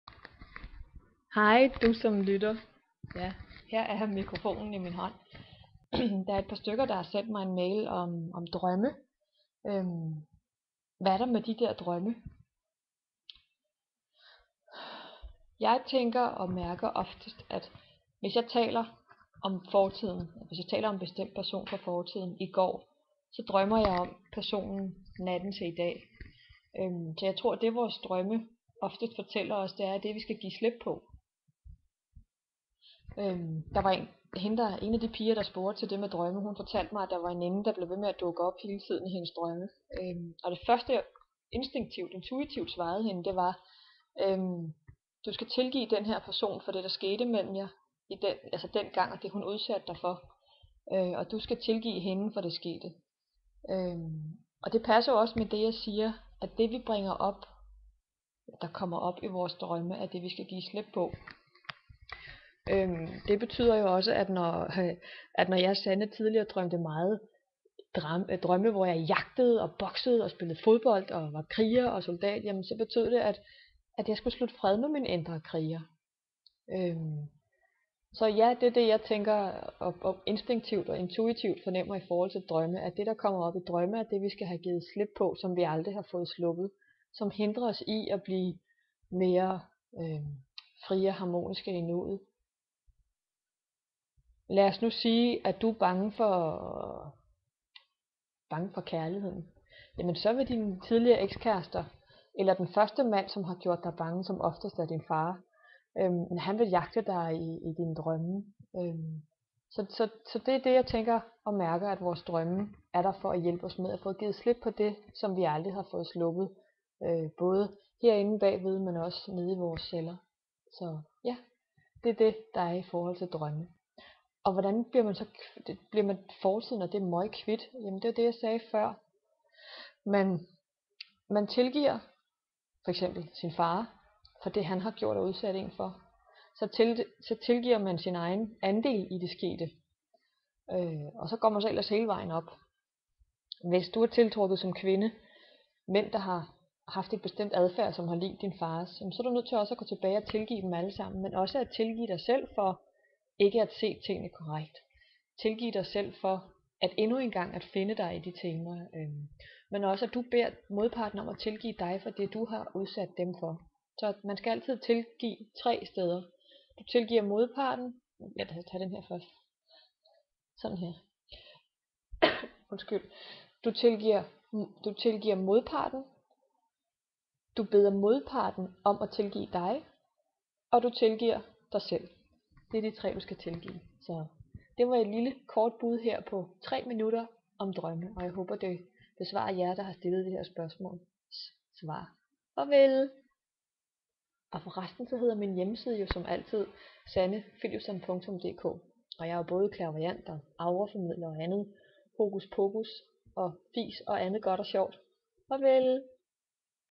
Video om drømme ♥
NØJ lyden er lav her - beklager.

En video om hvorfor vi drømmer det vi gør ♥